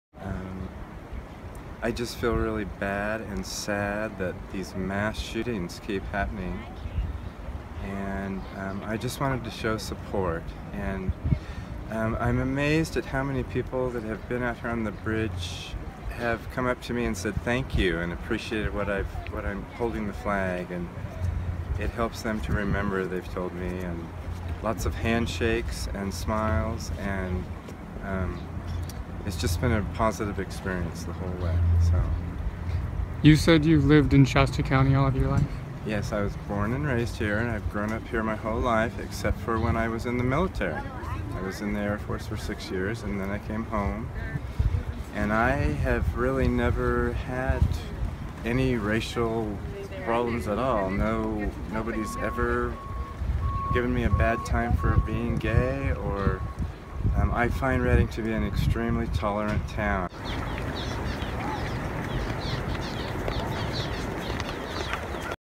Pride at the Sundial Bridge
Brad Hart walked the Sundial Bridge on Sunday afternoon to honor the victims of the Orlando, Fla. nightclub shooting.